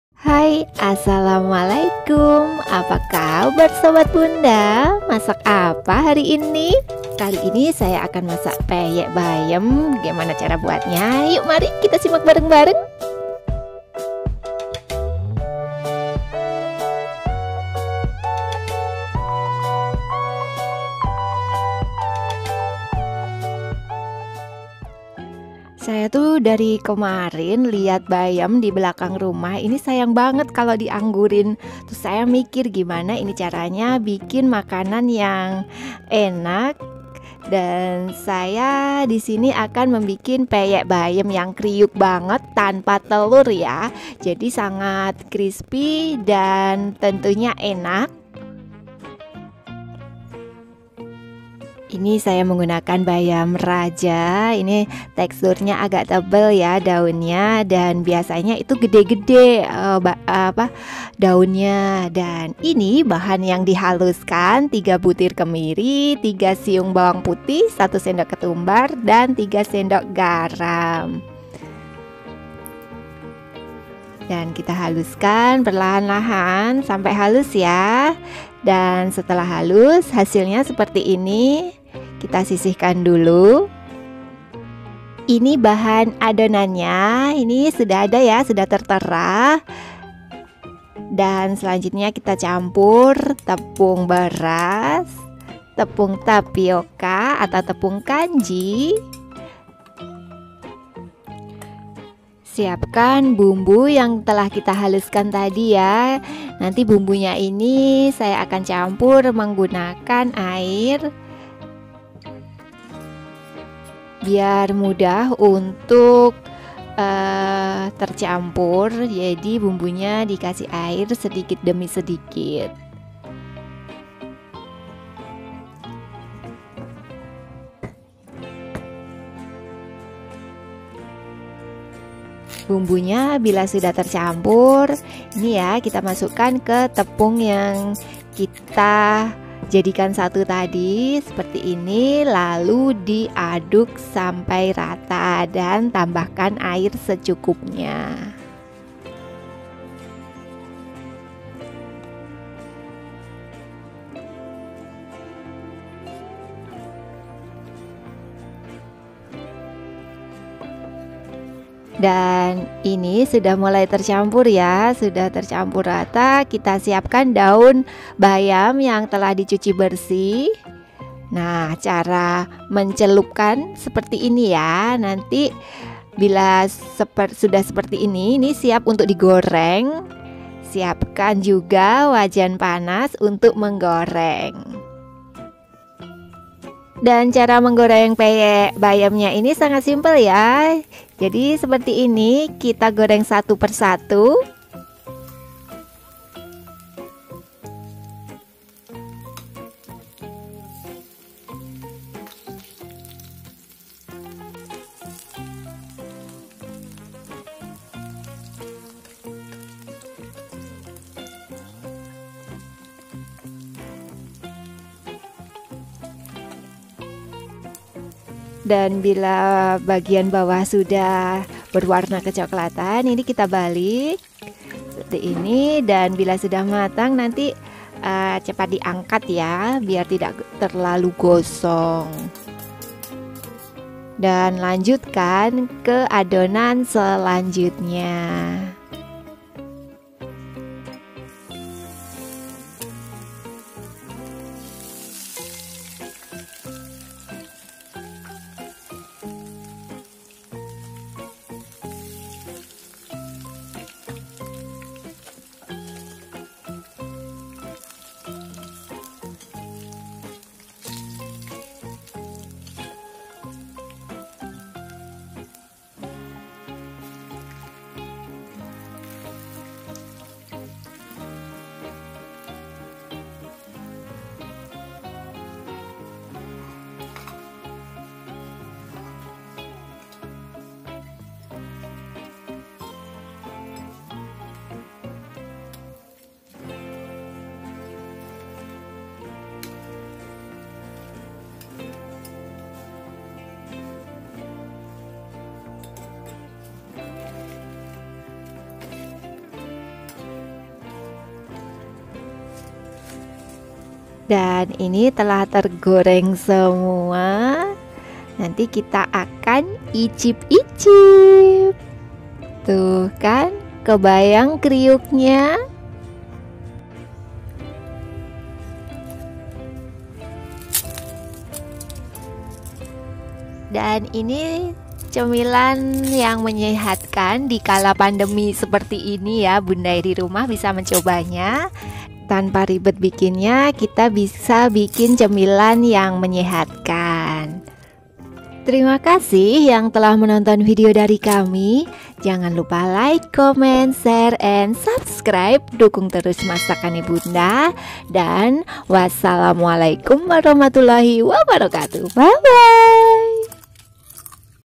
Cara Mudah Membuat Peyek Bayam Yang Gurih Dan Kriuk
langsung aja di simak videonya ya gaess cara membuat peyek bayam yang gurih dan krispi